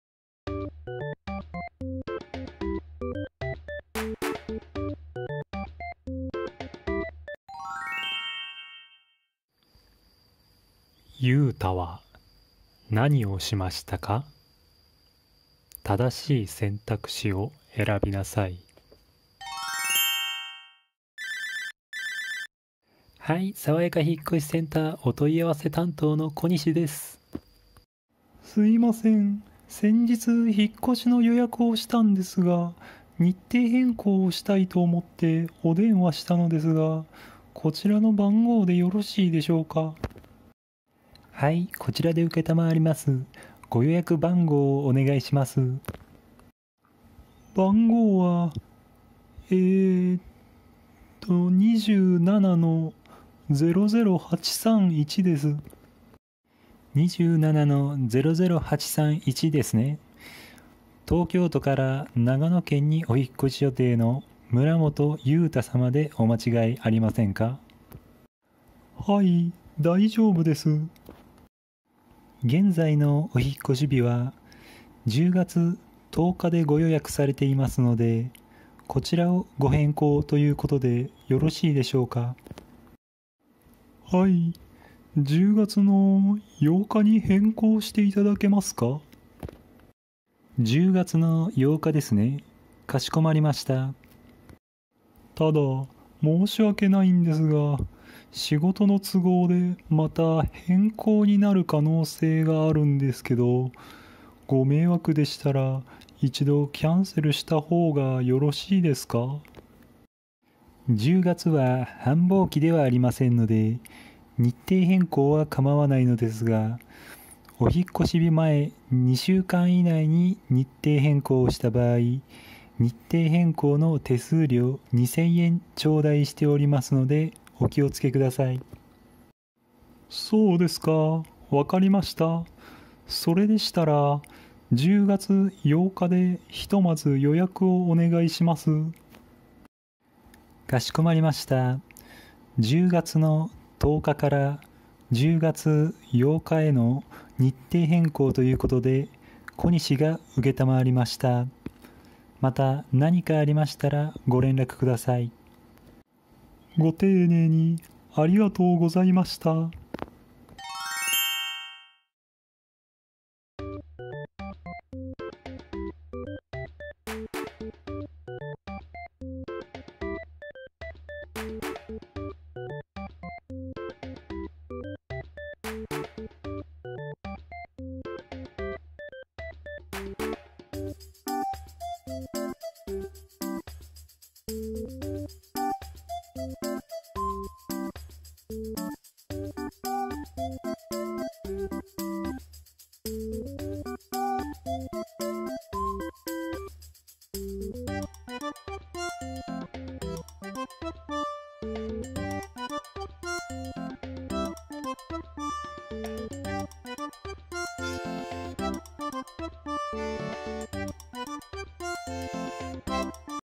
聴解 Choukai Listening 【JLPT N2～N1 level】電話 Phone Call Business Keigo 敬語 lesson
敬語 is important.

引越し→moving
日程変更→change date
予約→reservation
手数料→fee
もらう→(keigo)→頂戴する
繁忙期→busy season
現在→今→now
キャンセル→cancel
承りました≒了承しました≒引き受けました

8日(ようか)
9日(ここのか)
10日(とうか)